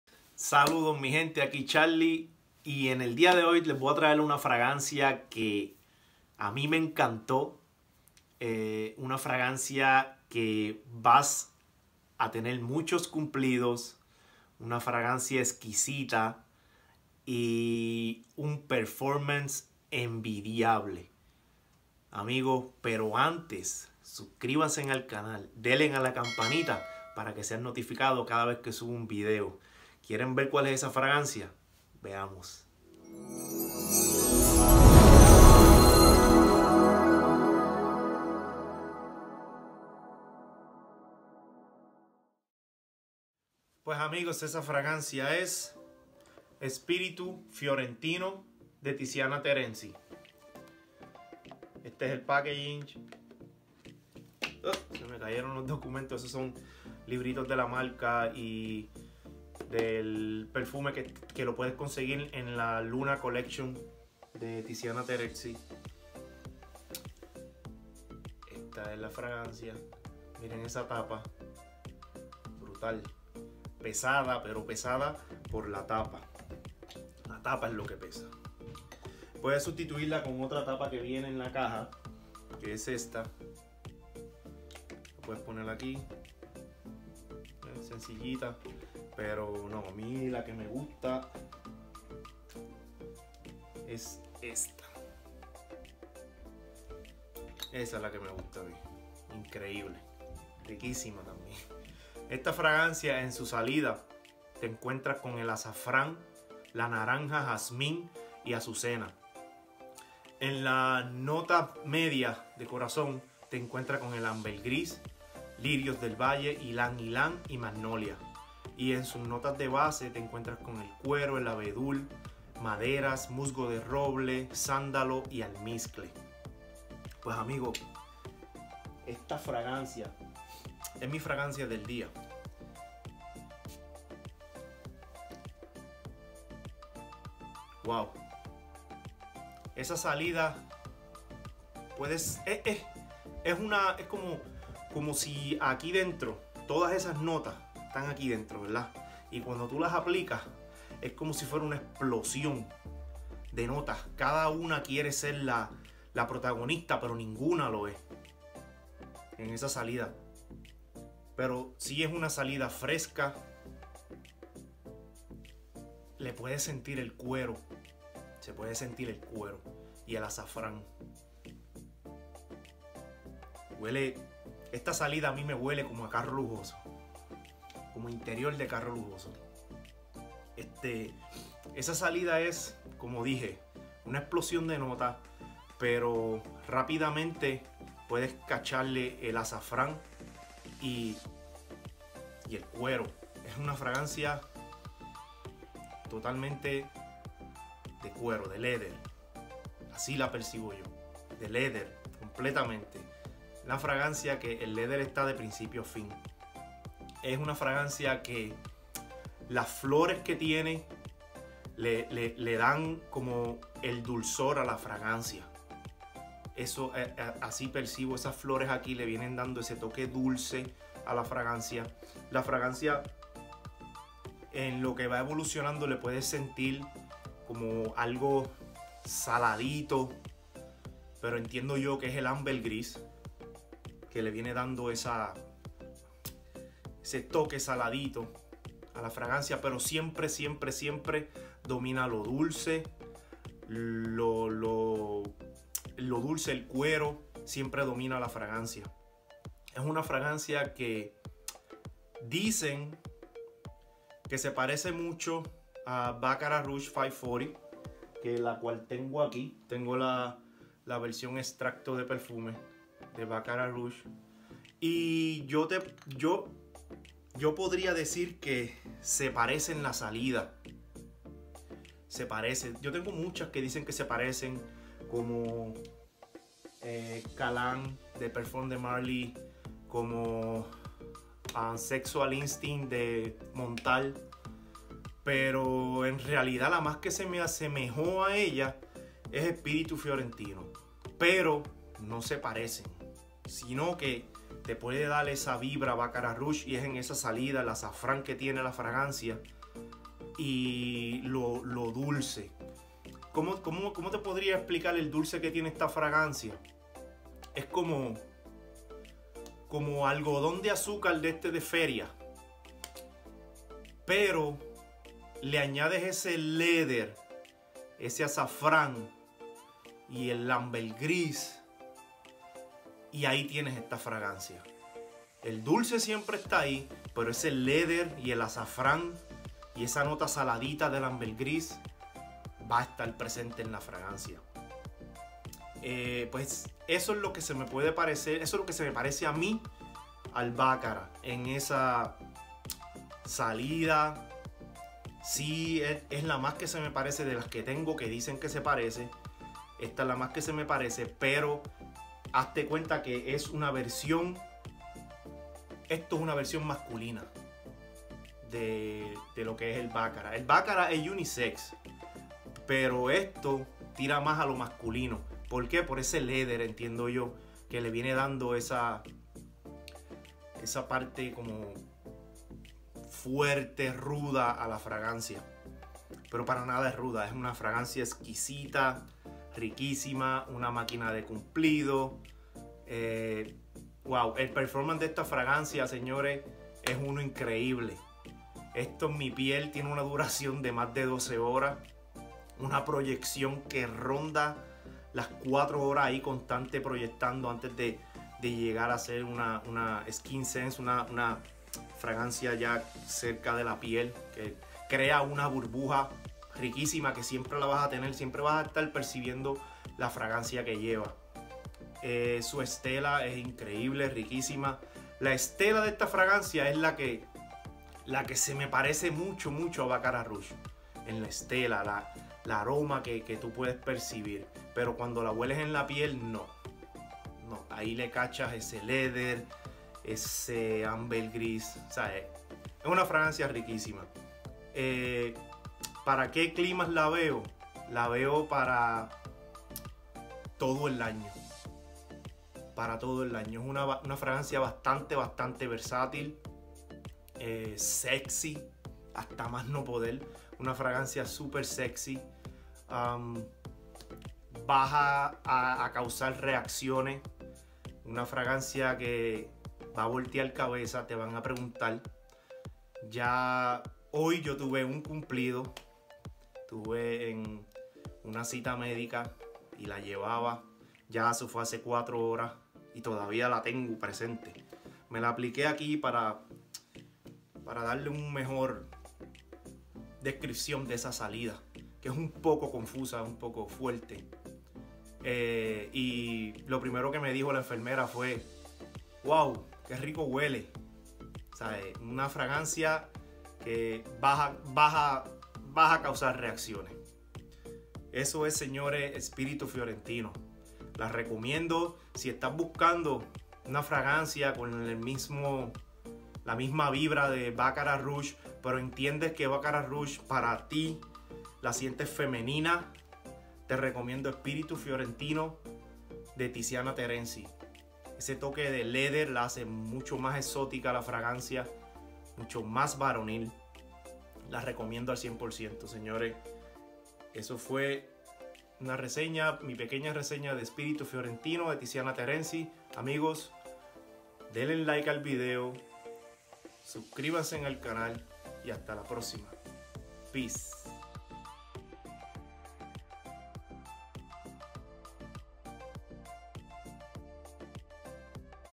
Spirito Fiorentino Una Delicia de Fragancia Con una Aroma exótica y un Performance Brutal (Español)
Esta Fragancia Se encuentra en la Luna Collection de Tiziana Terenzi Esta Fragancia tiene una similitud con BR540 pero esta es mucho más masculina trátenla les va encantar su aroma